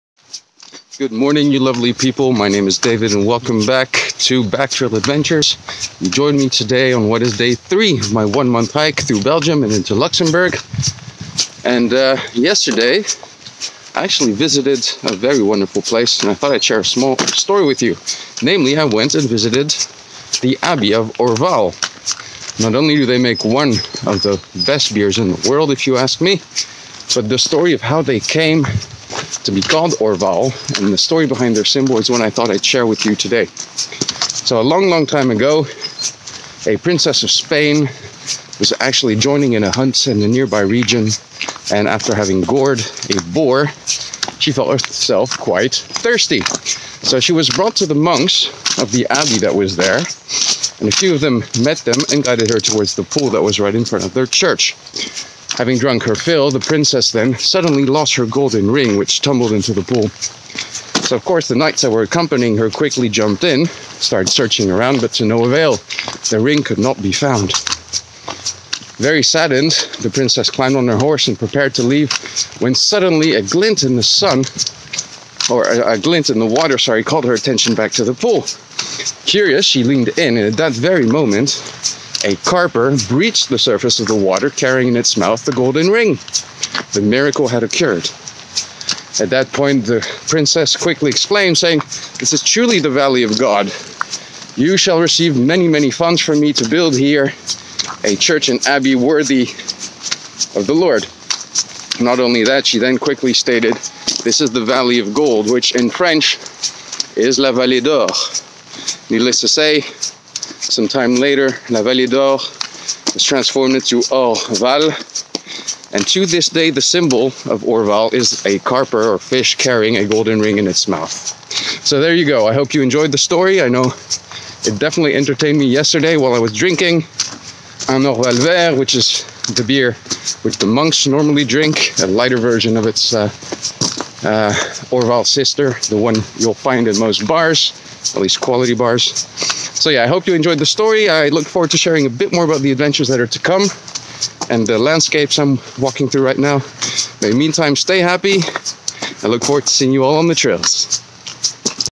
Storytime! The Story Of Orval
Day 2 of my hike brought me to the abbey of Orval where I not only enjoyed a delicious lunch with a few of their namesake brews, I also learned the story behind their name and emblem.